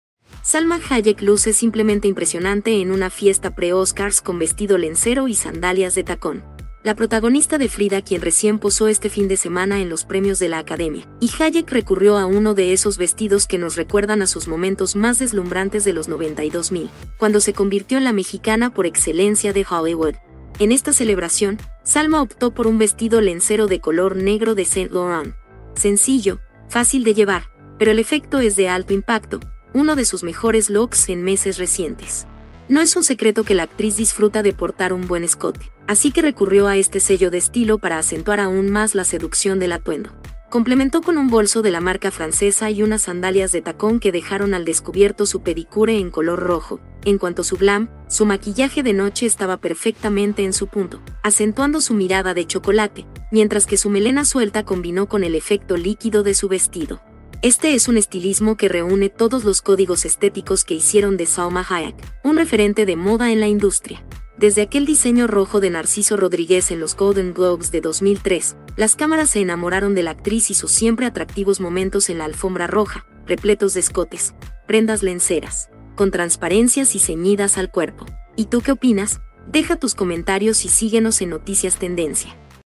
Salma Hayek luce simplemente impresionante en una fiesta pre Oscars con vestido lencero
Cuando Salma Hayek se lo propone, sabe muy bien cómo rendir pleitesía a la seducción y su última aparición dentro del marco de los premios Oscar acaba de confirmarlo.